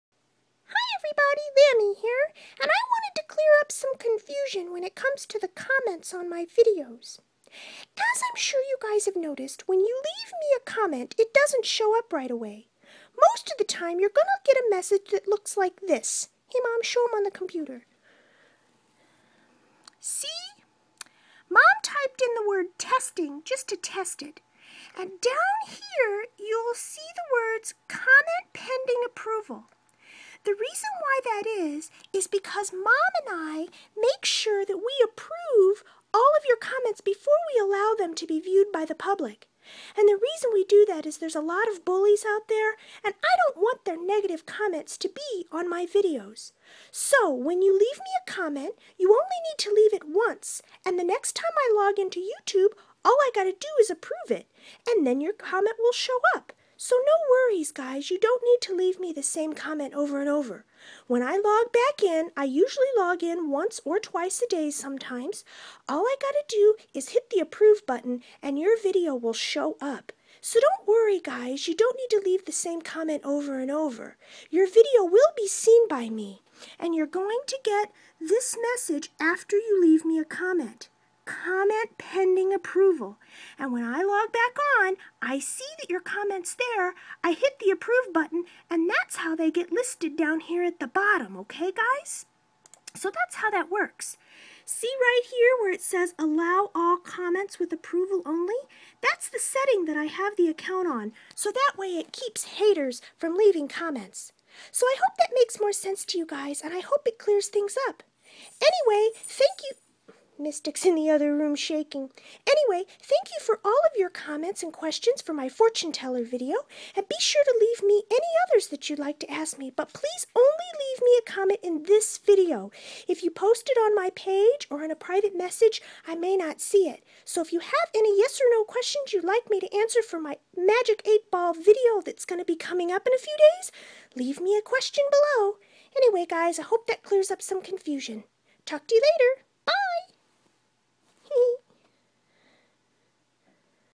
#119: Your Comments... - LambCam
I wanted to explain you guys why your videos do not always show up right away when you post me one. There seems to be some confusion...